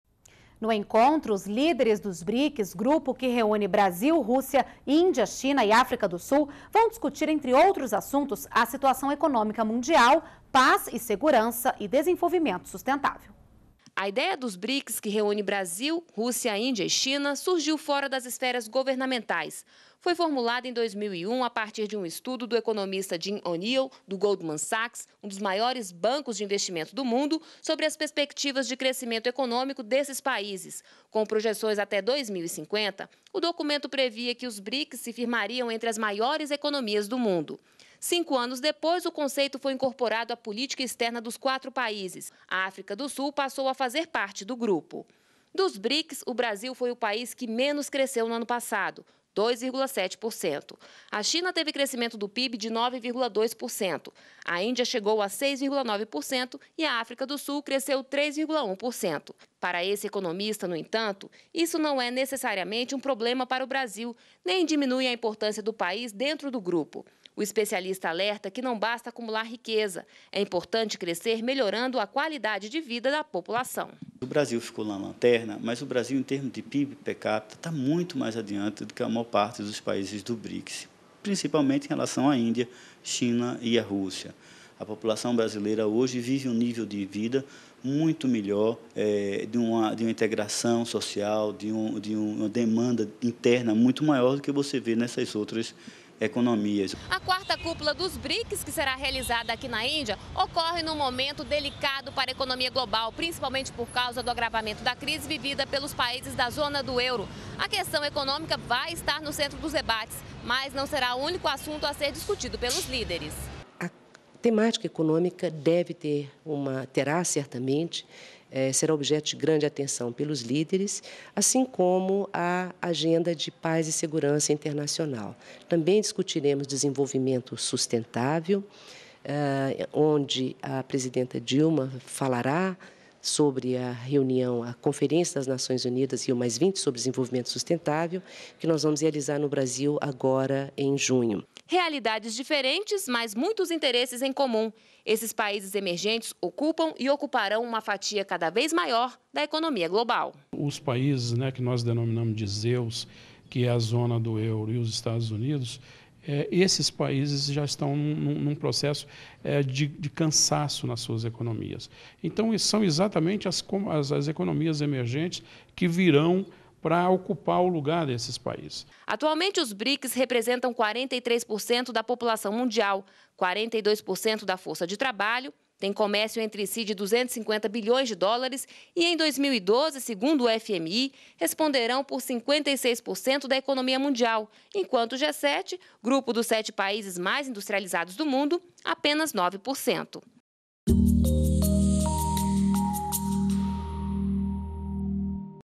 Dilma Rousseff deve falar sobre a Rio + 20 na 4ª Cúpula do Brics
NBR NOTÍCIAS - 27.03.12: A 4ª Cúpula do Brics - formado pelos países emergentes Brasil, Rússia, Índia, China e África do Sul - vai tratar de diversos assuntos, como a situação econômica mundial, paz e segurança, e desenvolvimento sustentável.  No encontro, a presidenta Dilma Rousseff deve falar sobre a Conferência das Nações Unidas Rio +20, que será realizada no Brasil em junho deste ano.